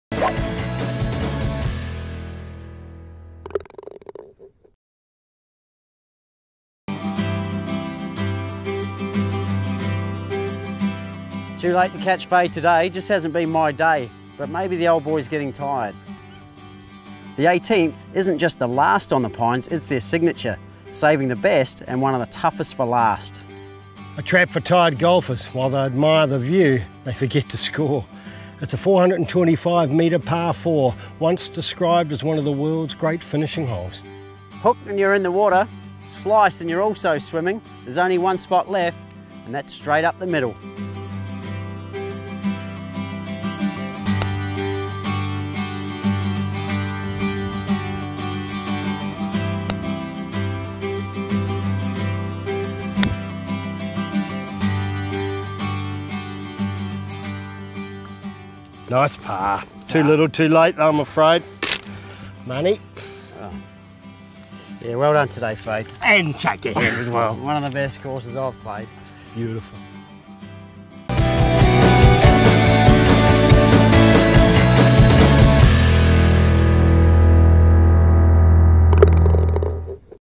Sanctuary Cove The Pines Signature Hole
Golf Getaway's Mirror and Fade take on Sanctuary Cove The Pines Hardest and Signature Hole, the Par 4 18th. See how they go?